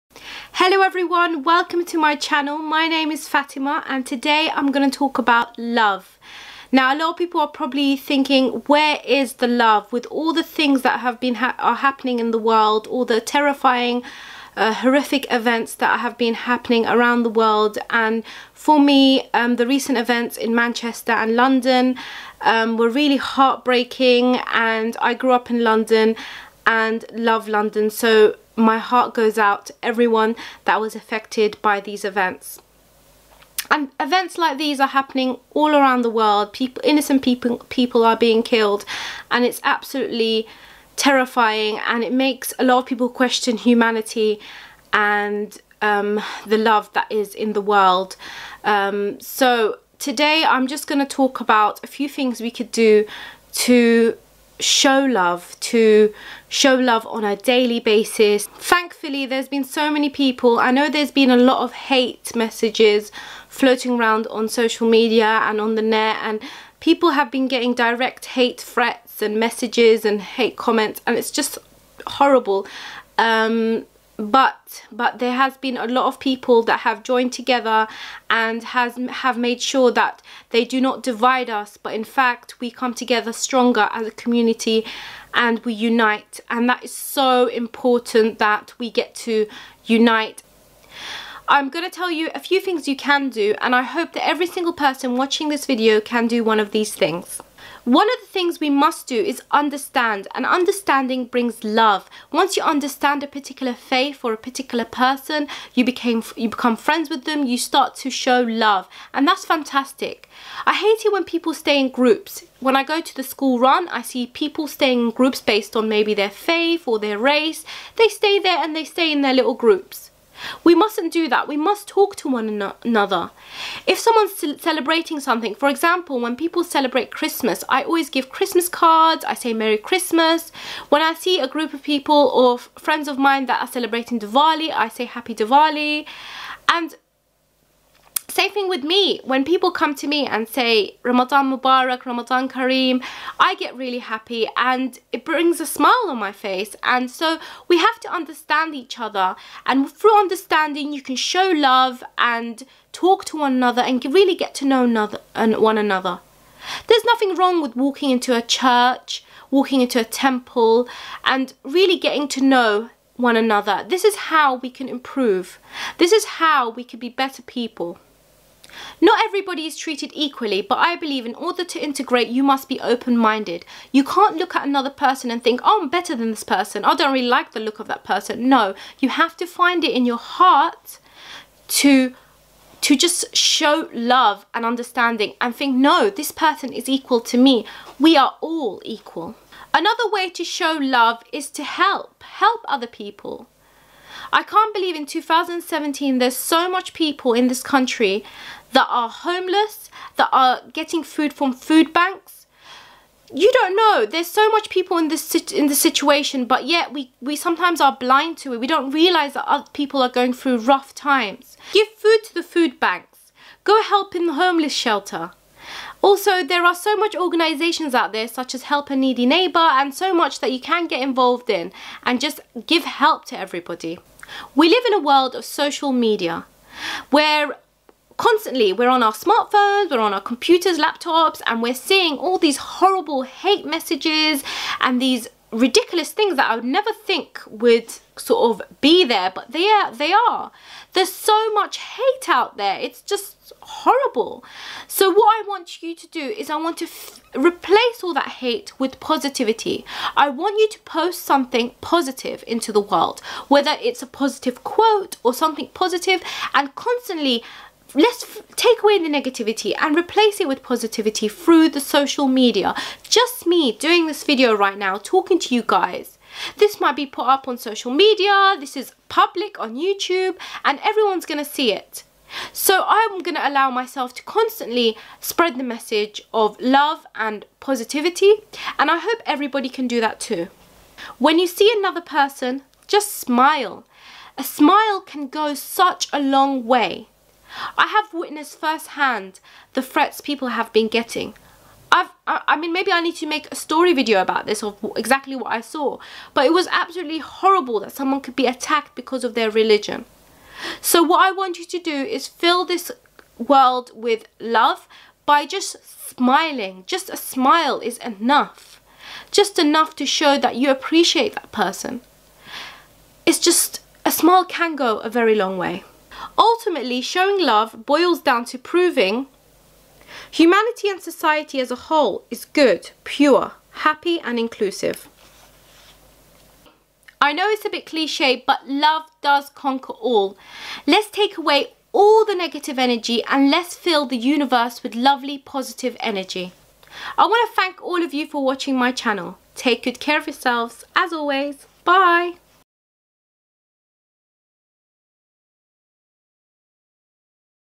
Where is the Love? | Anything Fatema | Fatema's Art Show
Where is the Love? Everyone is thinking this. I go through different ways we can all show love. I feel like its such a powerful message that I needed to share with all of you.
After recent events I have had to think about humanity and I want to spread the word of Love and help make this world a better place.